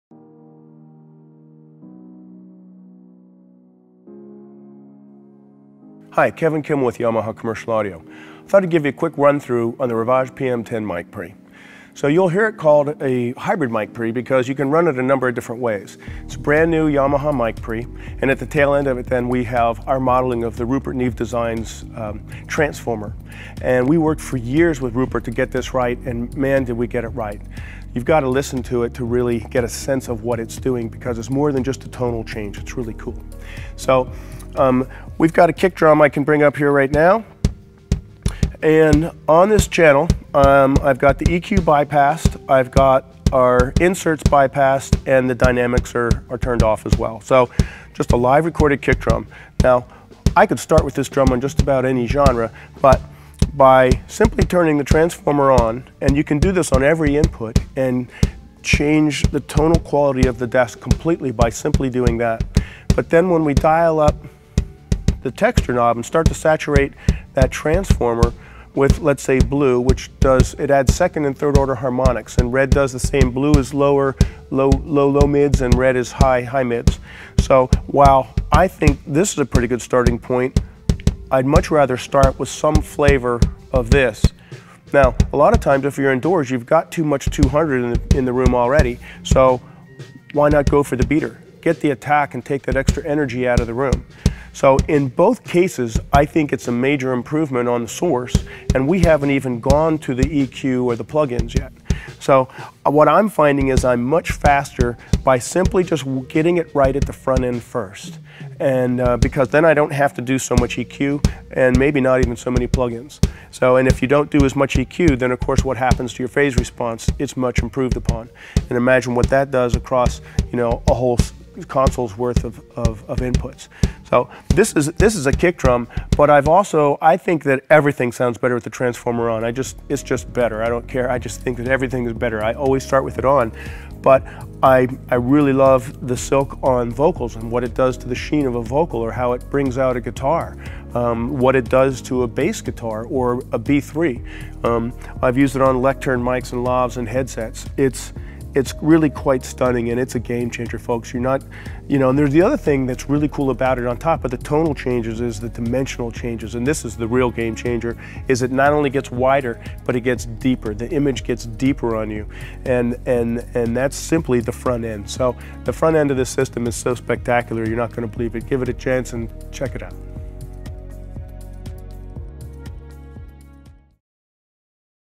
Yamaha RIVAGE PM10: Hybrid Mic Preamps
Kevin Kimmel, Mixer Product Manager at Yamaha Corporation of America, introduces the Rupert Neve Designs transformer emulation and Silk processing incorporated into the new hybrid mic preamps of Yamaha RIVAGE PM10, and explains how easily it makes not only a tonal change but a cool dimensional change.